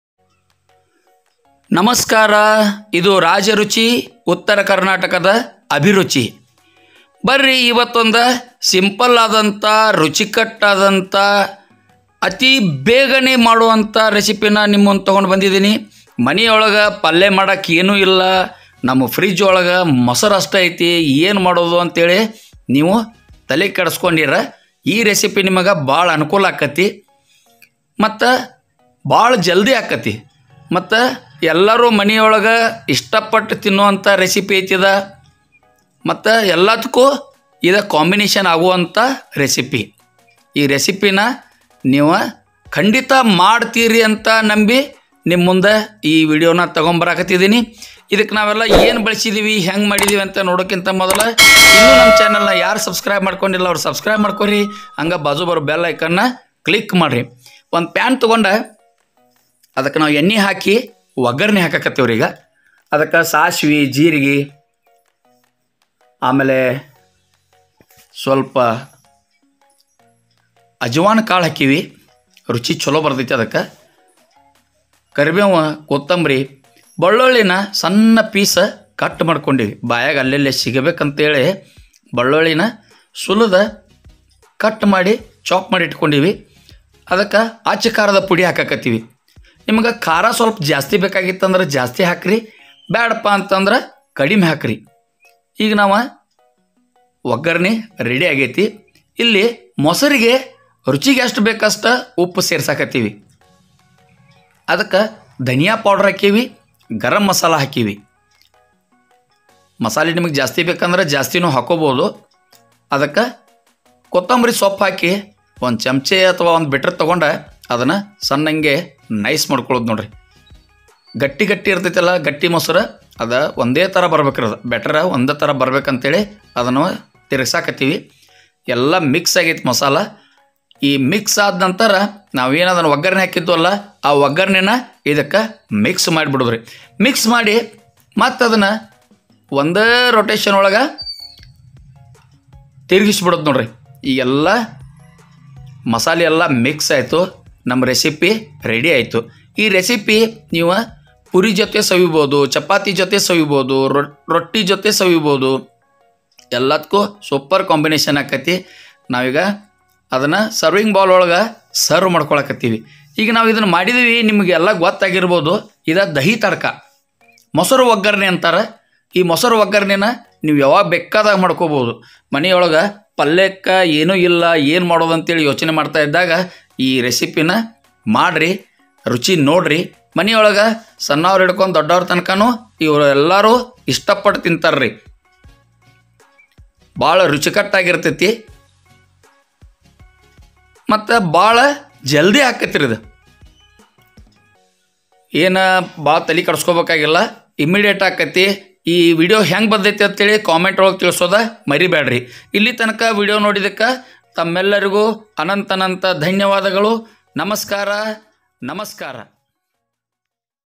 ಮನೆಯಲ್ಲಿ ತರಕಾರಿ ಖಾಲಿ ಆಗಿದೆಯಾ? ಮೊಸರು ಇದ್ರೆ ಸಾಕು ಇಂಥ ರುಚಿಯಾದ ರೆಸಿಪಿ ರೆಡಿಯಾಗುತ್ತೆ | testy new recipe..
ಮನೆಯಲ್ಲಿ ತರಕಾರಿ ಖಾಲಿ ಆದಾಗ ಮೊಸರು ಇದ್ರೆ ಸಾಕು, ಇಂಥ ಹೊಸ ರುಚಿಯ ಅಡುಗೆ ಮಾಡಿ ಮನೆ ಮಂದಿ ಎಲ್ಲ ಇಷ್ಟಪಡುವವರು.